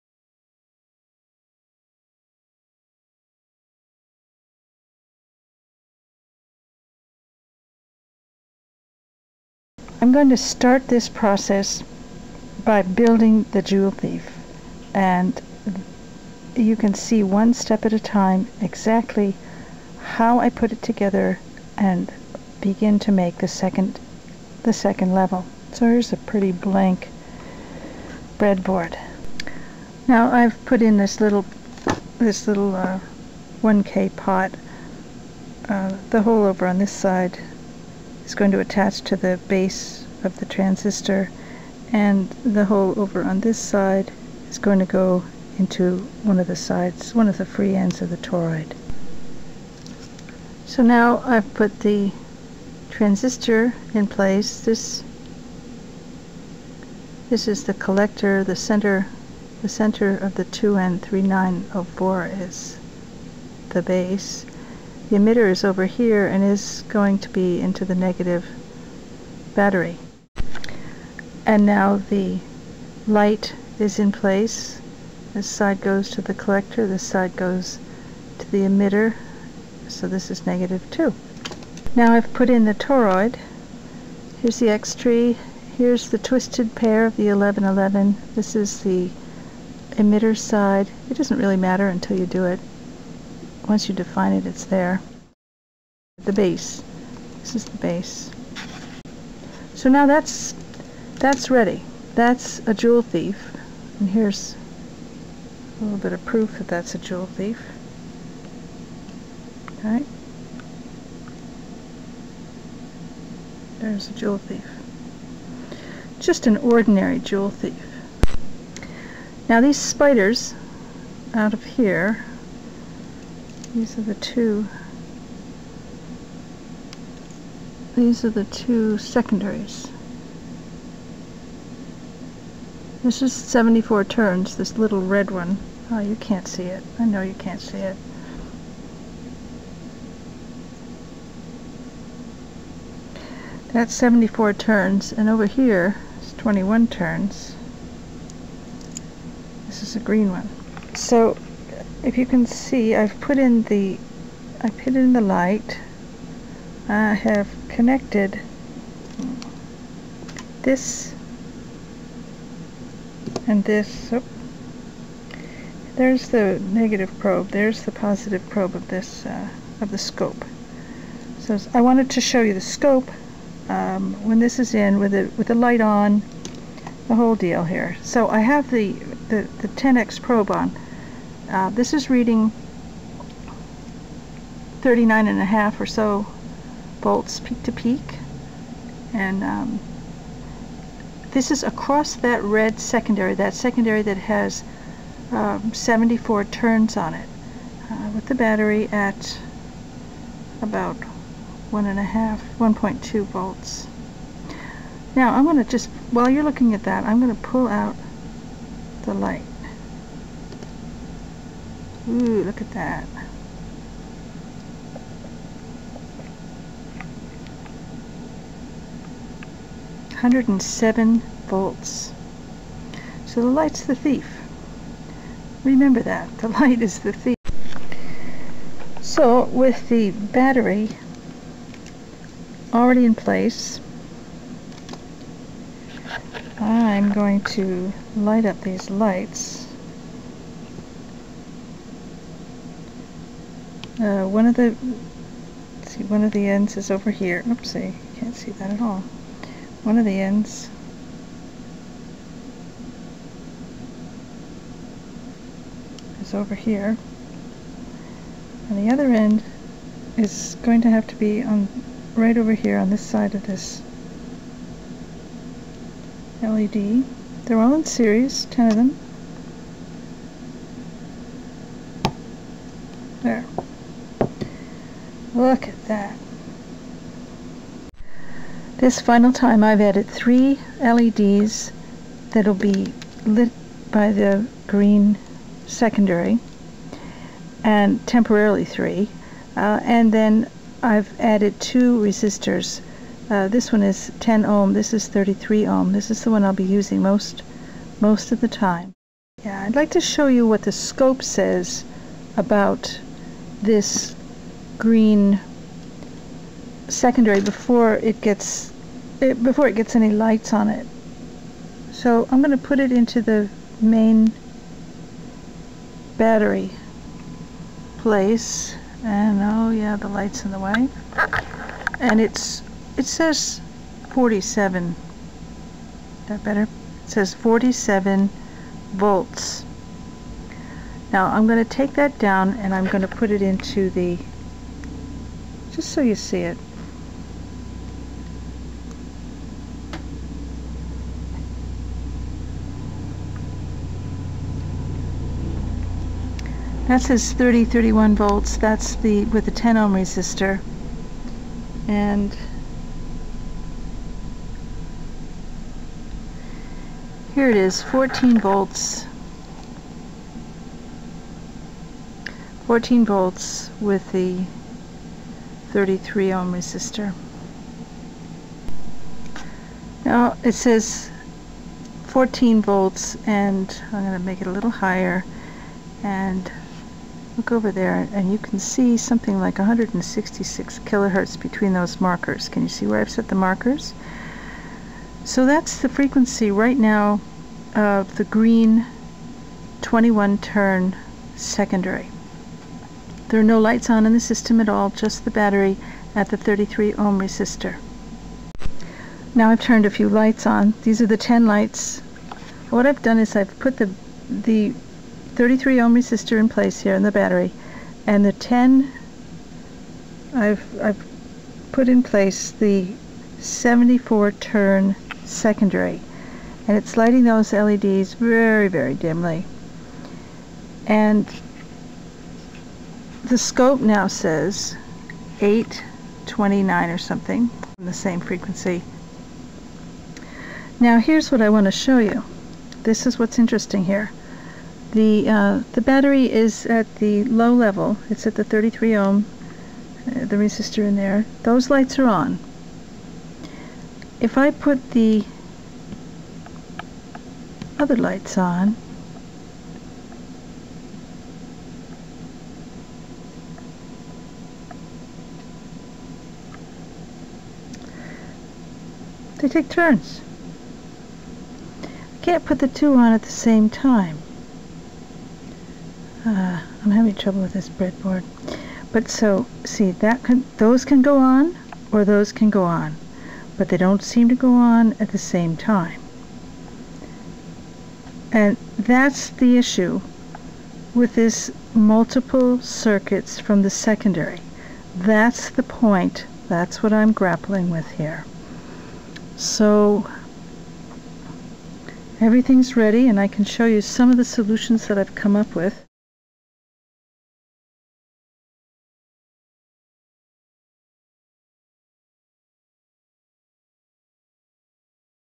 Joule Thief with 2 Secondaries Explanation Part 3
In part 3, I begin with a fresh breadboard and build this joule thief with 2 secondaries. I show some scope shots of the output from each secondary with various lights and resistors. Finally I show the major difficulty in working a circuit of this kind.